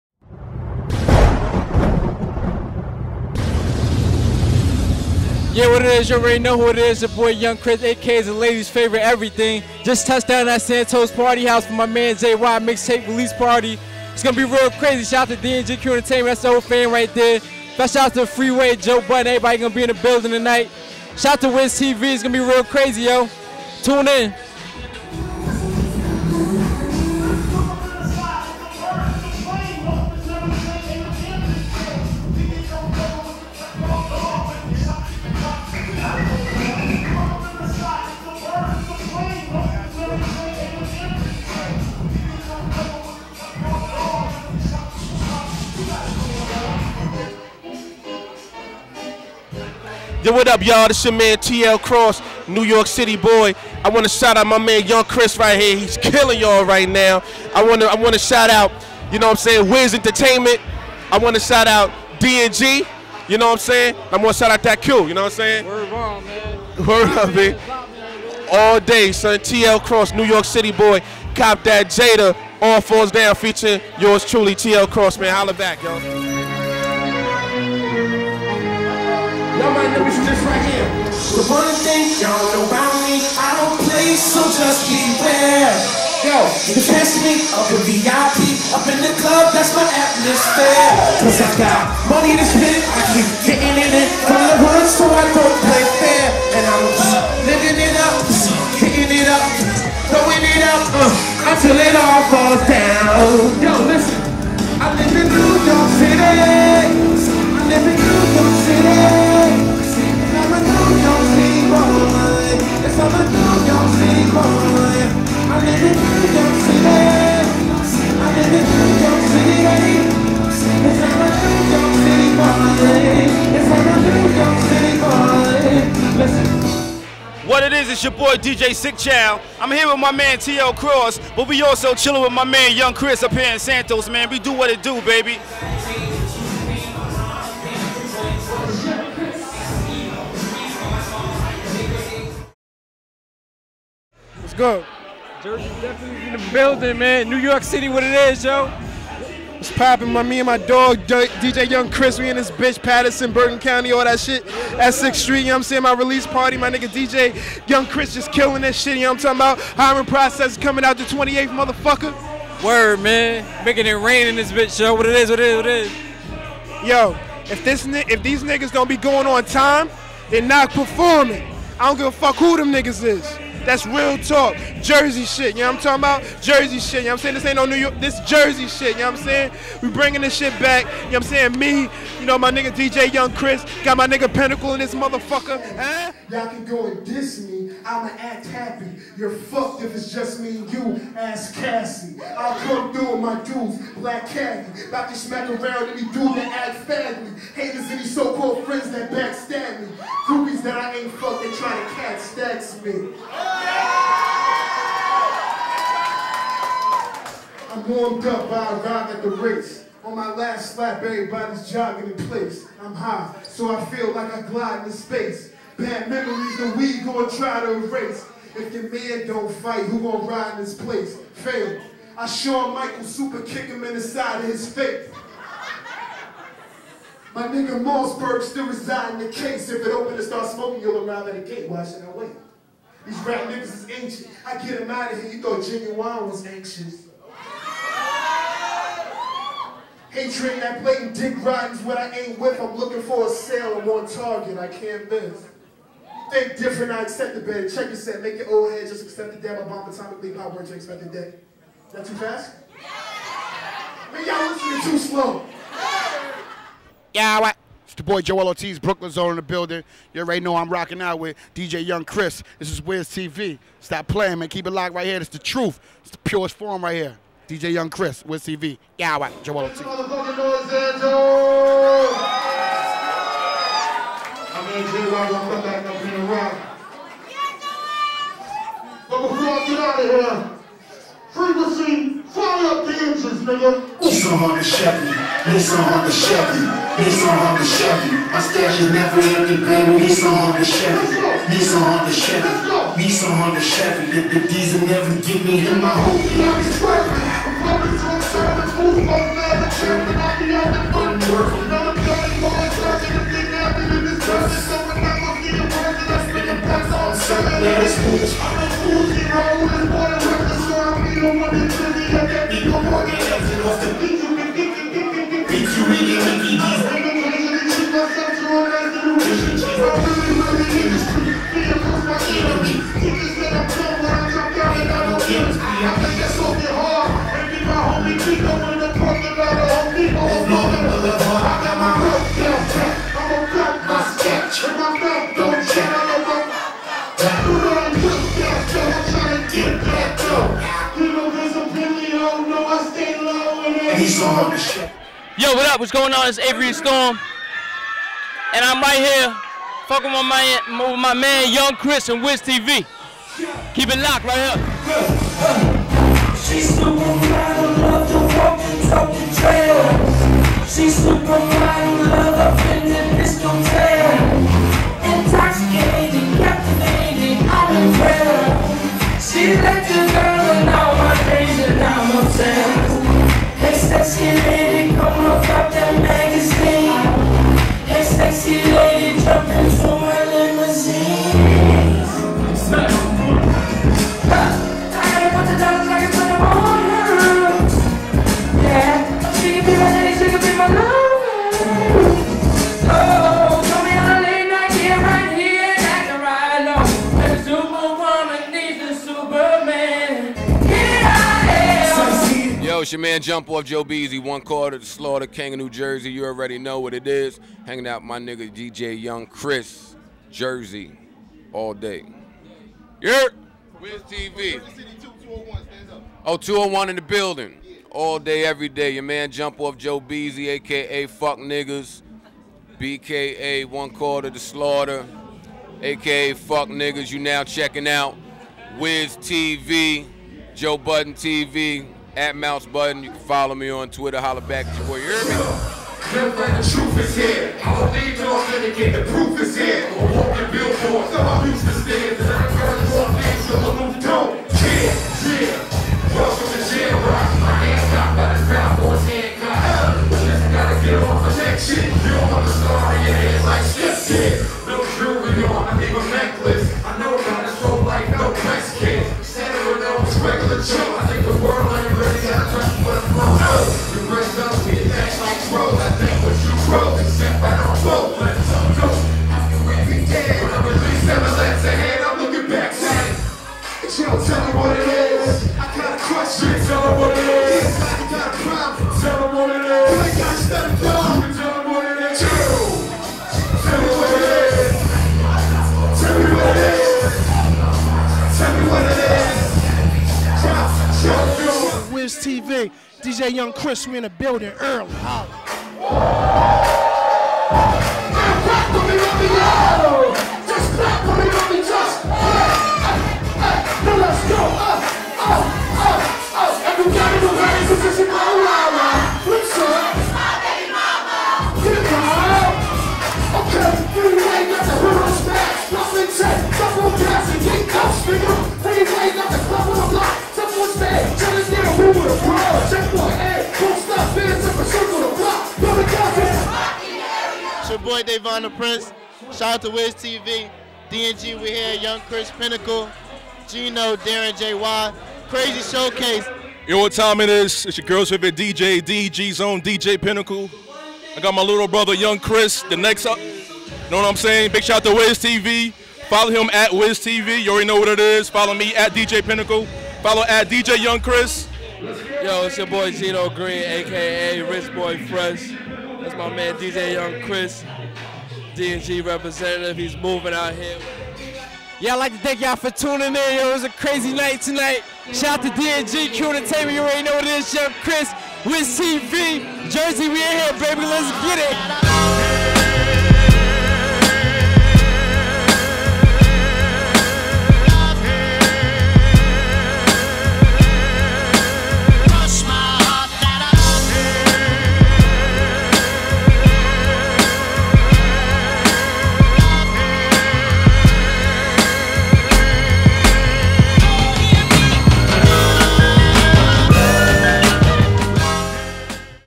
Joe Budden Joel Ortiz Freeway & Avery storm Perform at Dj Young Chris Jy show case
check out joe budden performing live in nyc